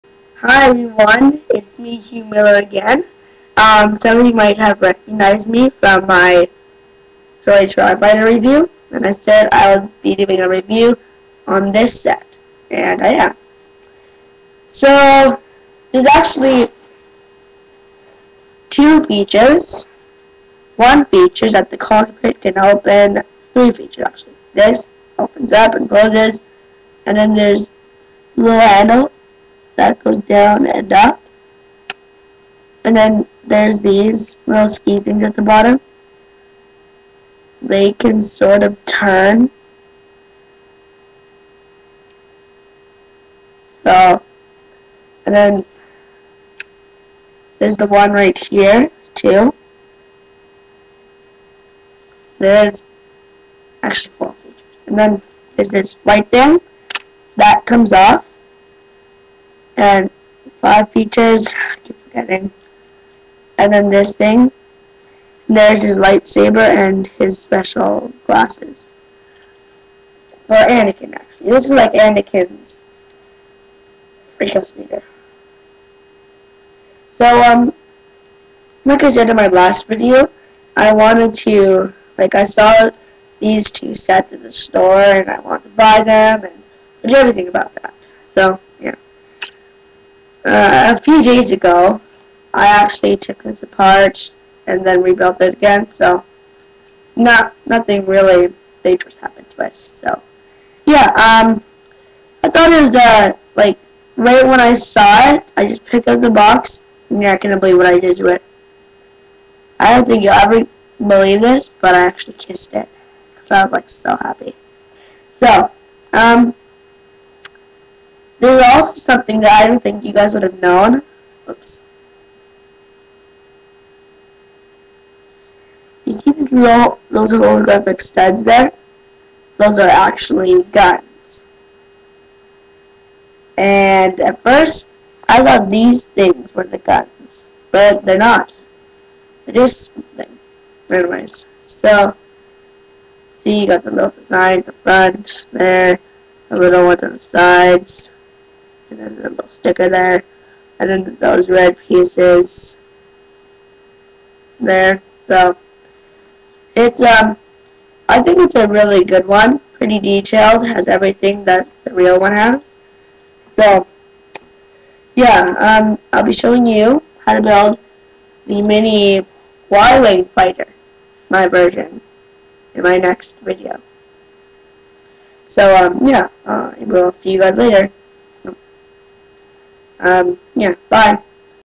Star Wars Lego Freeco speeder review
Huw shows a review on the new 2010 freeco speeder.